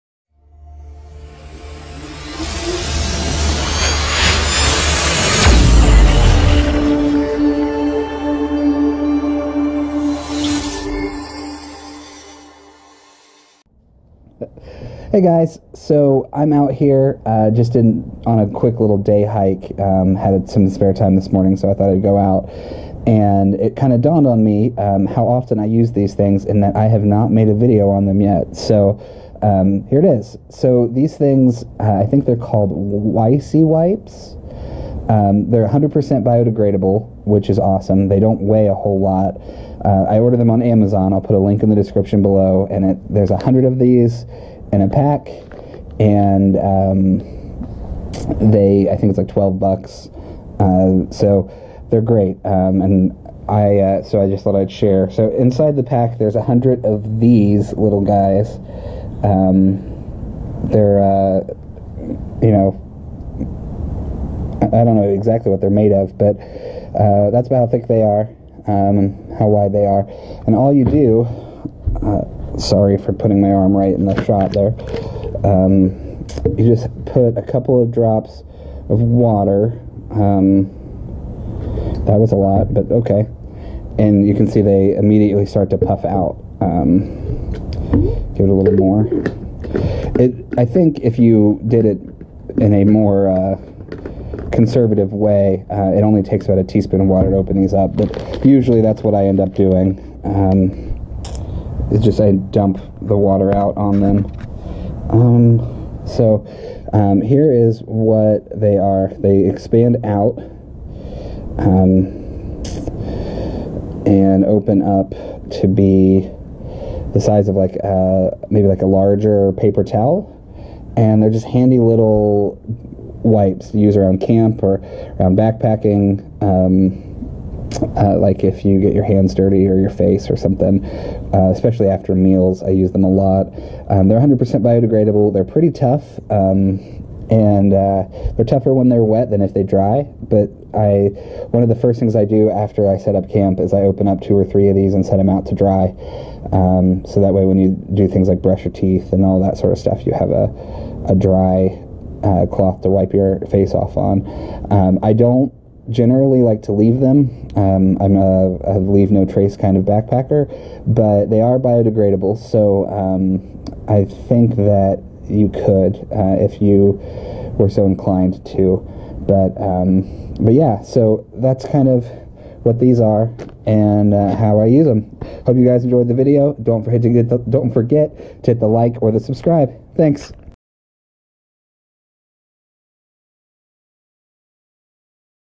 Expanding Napkin/hand towel For Moms And Backpackers
These are cool expanding towels that easy to throw into your backpack or purse. Great for Bushcraft/Trailcraft, Camping, preppers, and mom's with messy kids! I highly recomend these towels, sometimes called aqua coins.

I ordered these off of Amazon; Link

Shot with my GoPro Hero 5 and it's on board audio (sorry). All post editing done in Filmore.